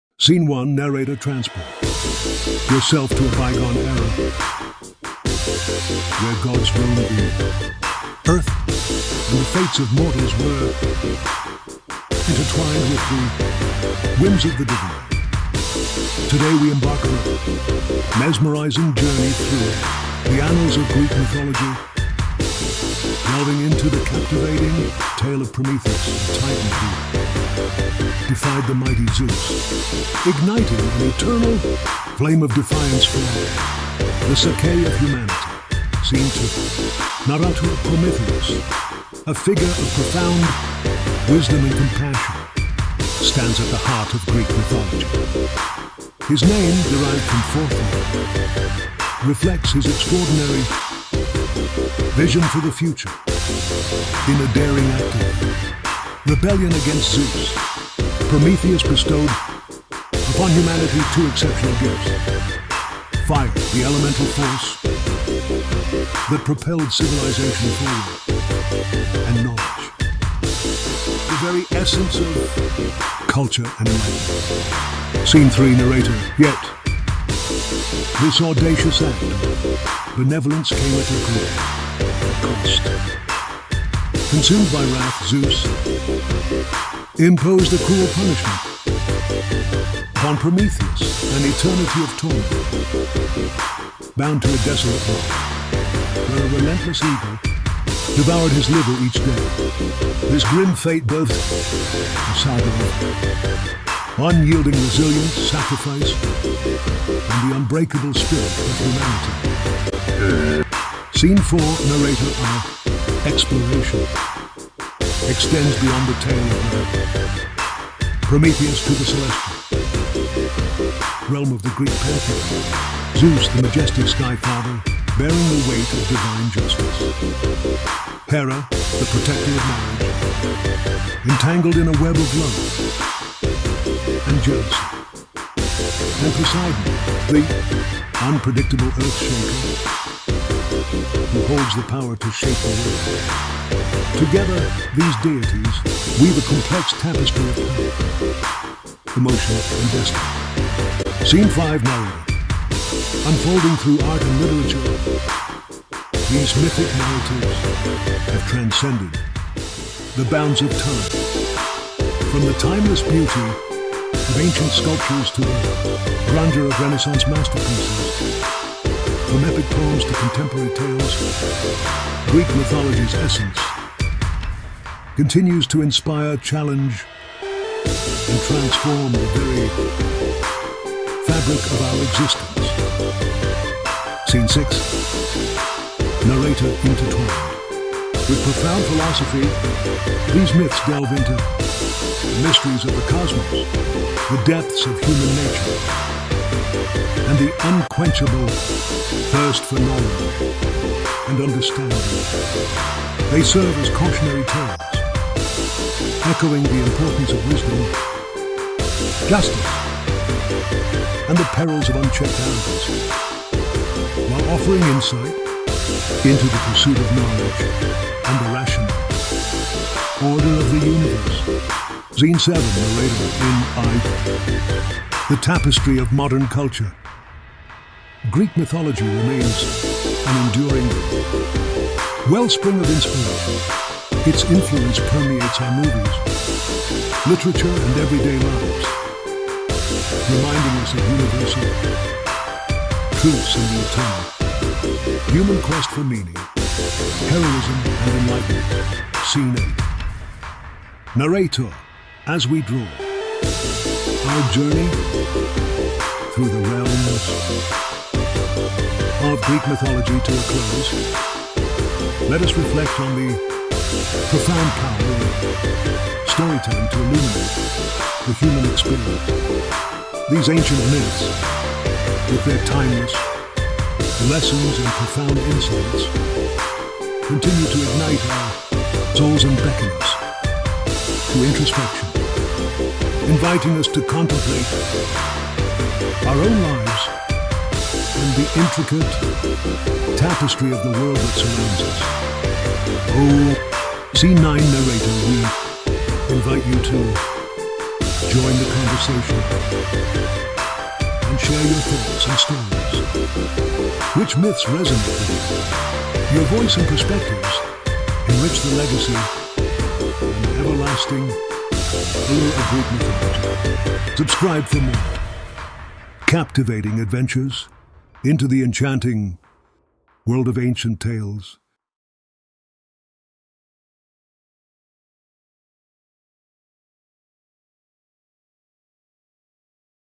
The Eternal Flame of Prometheus: Exploring Greek Mythology
Dive into the captivating world of Greek mythology with the tale of Prometheus and the pantheon of gods. Explore their enduring influence on culture, art, literature, and philosophy.